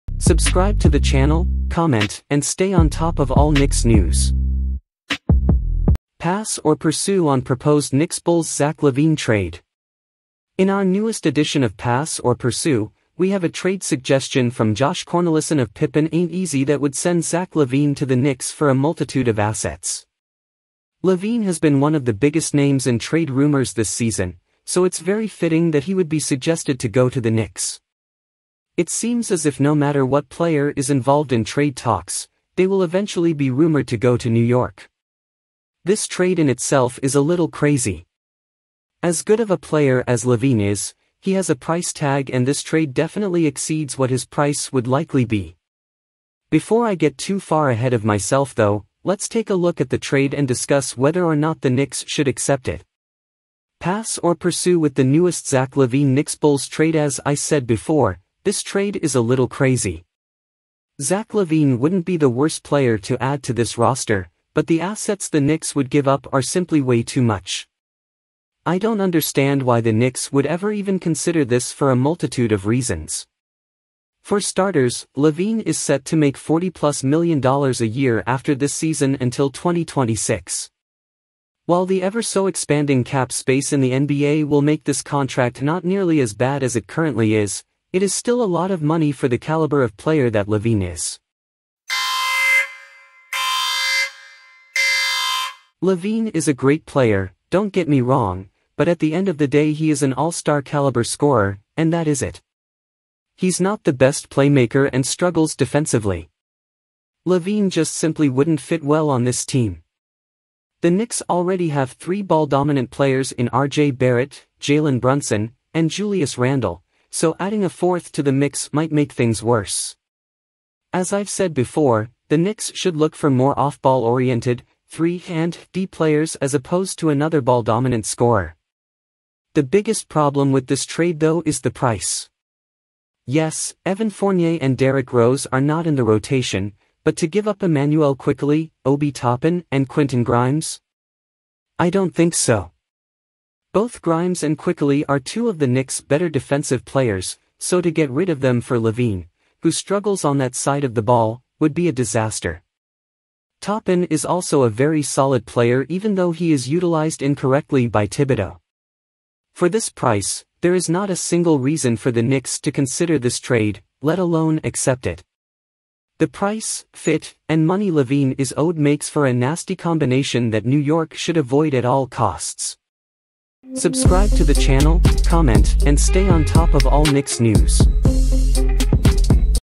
🏀🔥the change the fans wanted!!! knicks fans- knicks news- knicks trade rumors-knicks rumors
Pass or Pursue on proposed Knicks-Bulls Zach LaVine trade

In our newest addition of Pass or Pursue, we have a trade suggestion from Josh Cornelissen of Pippen Ain’t Easy that would send Zach LaVine to the Knicks for a multitude of assets.

LaVine has been one of the biggest names in trade rumors this season, so it’s very fitting that he would be suggested to go to the Knicks. It seems as if no matter what player is involved in trade talks, they will eventually be rumored to go to New York.

This trade in itself is a little crazy. As good of a player as LaVine is, he has a price tag and this trade definitely exceeds what his price would likely be. Before I get too far ahead of myself though, let’s take a look at the trade and discuss whether or not the Knicks should accept it.

Pass or Pursue with the newest Zach Lavine Knicks-Bulls tradeAs I said before, this trade is a little crazy. Zach LaVine wouldn’t be the worst player to add to this roster, but the assets the Knicks would give up are simply way too much.

I don’t understand why the Knicks would ever even consider this for a multitude of reasons. For starters, LaVine is set to make $40+ million a year after this season until 2026. While the ever-so-expanding cap space in the NBA will make this contract not nearly as bad as it currently is, it is still a lot of money for the caliber of player that LaVine is.

LaVine is a great player, don’t get me wrong, but at the end of the day he is an All-Star caliber scorer, and that is it. He’s not the best playmaker and struggles defensively. LaVine just simply wouldn’t fit well on this team.

The Knicks already have three ball-dominant players in RJ Barrett, Jalen Brunson, and Julius Randle, so adding a fourth to the mix might make things worse. As I’ve said before, the Knicks should look for more off-ball oriented, 3-and-D players as opposed to another ball-dominant scorer.

The biggest problem with this trade though is the price. Yes, Evan Fournier and Derrick Rose are not in the rotation, but to give up Immanuel Quickley, Obi Toppin, AND Quentin Grimes? I don’t think so.

Both Grimes and Quickley are two of the Knicks’ better defensive players, so to get rid of them for LaVine, who struggles on that side of the ball, would be a disaster. Toppin is also a very solid player even though he is utilized incorrectly by Thibodeau.

For this price, there is not a single reason for the Knicks to consider this trade, let alone accept it. The price, fit, and money LaVine is owed makes for a nasty combination that New York should avoid at all costs.